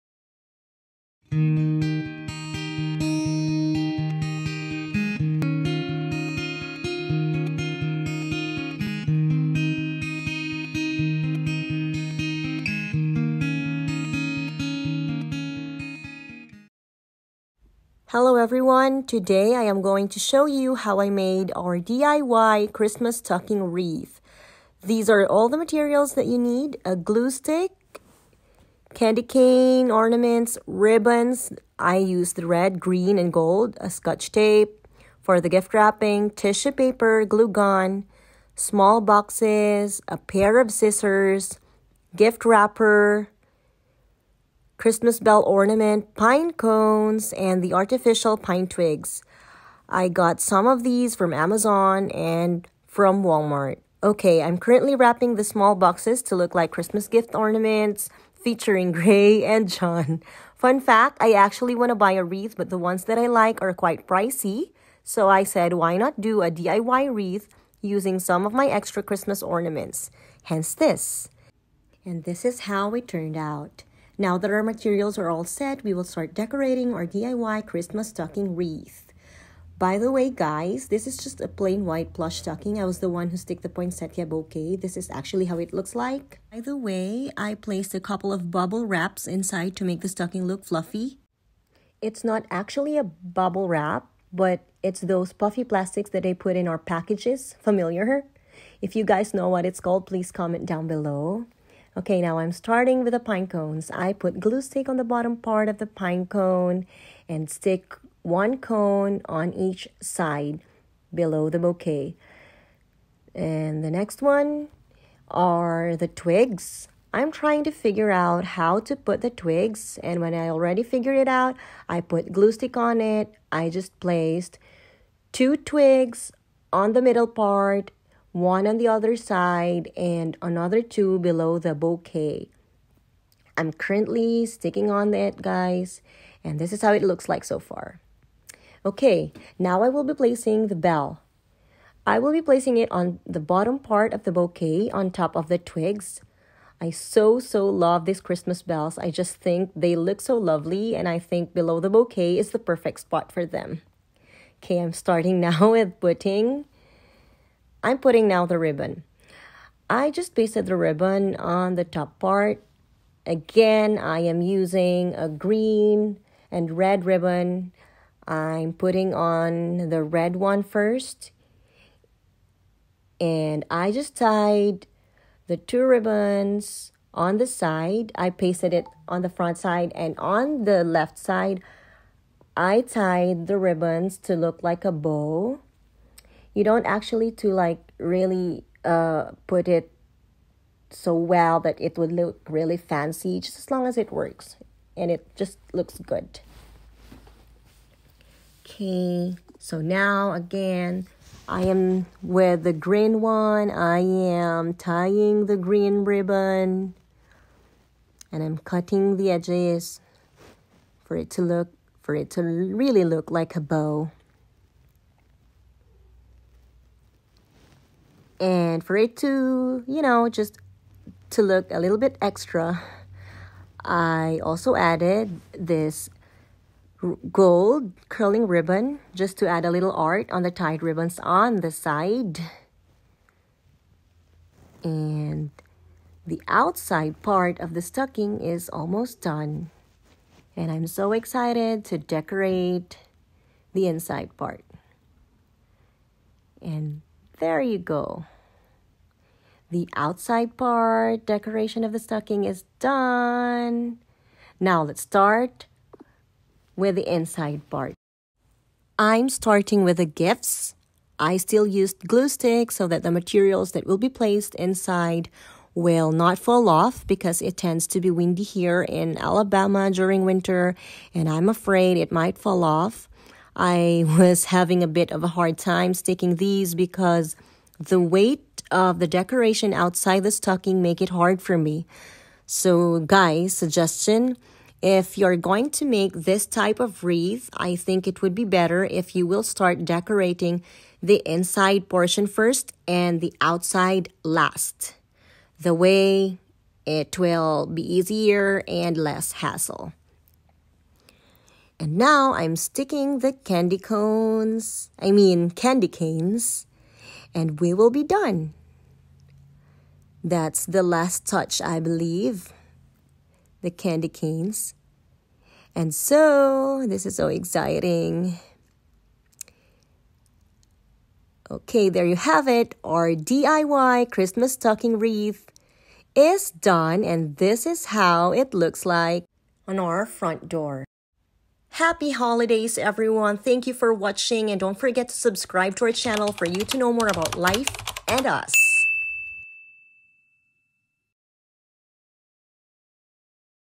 DIY CHRISTMAS STOCKING WREATH🎄🌟 (ASMR)
This is a DIY christmas stocking wreath. You can finish this in under 10 minutes if you just have all materials available.